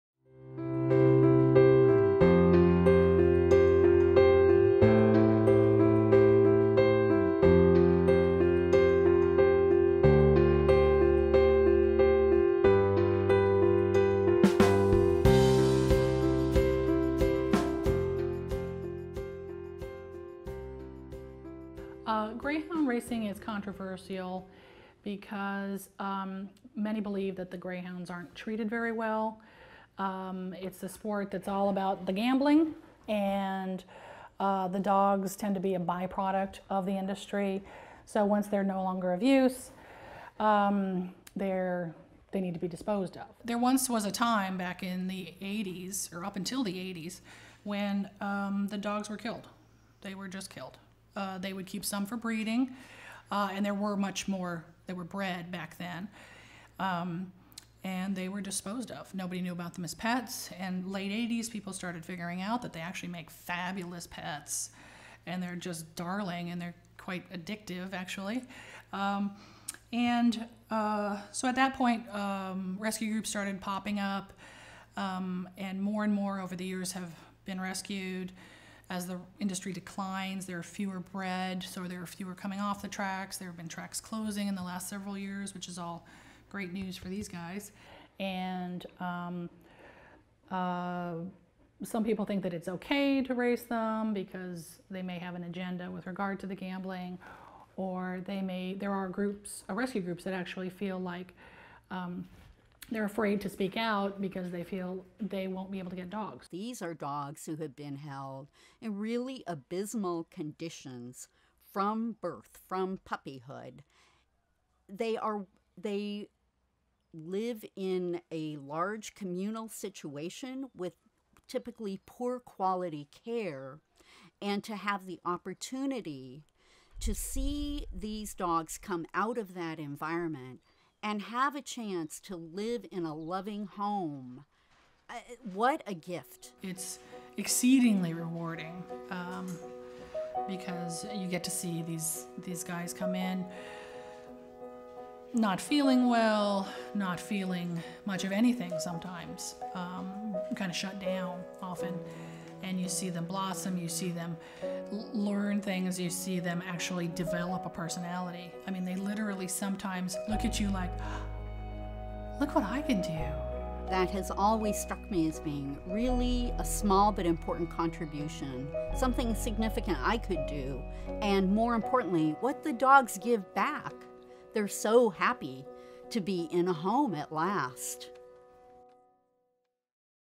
Nicholas Lawless Doc Teaser: Greyhound Rescue
This is a quick edit for my image post production class. I uploaded it to my channel because my online college server cannot handle the file size.